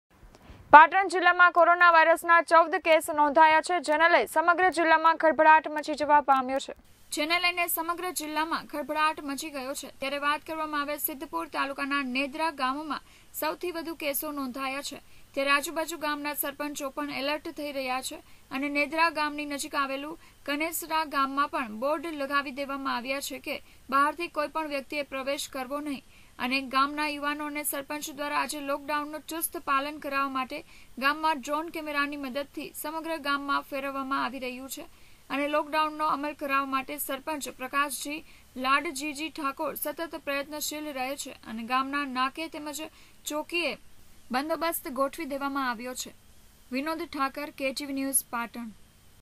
PATAN: પાટણ જિલ્લામાં કોરાના વાયરસના ૧૪ કેસ નોંધાયા છે,જેને લઈ સમગ્ર જિલ્લામાં ખળભળાટ
PATAN: પાટણ જિલ્લામાં કોરાના વાયરસના ૧૪ કેસ નોંધાયા છે,જેને લઈ સમગ્ર જિલ્લામાં ખળભળાટ મચી જવા પામ્યો.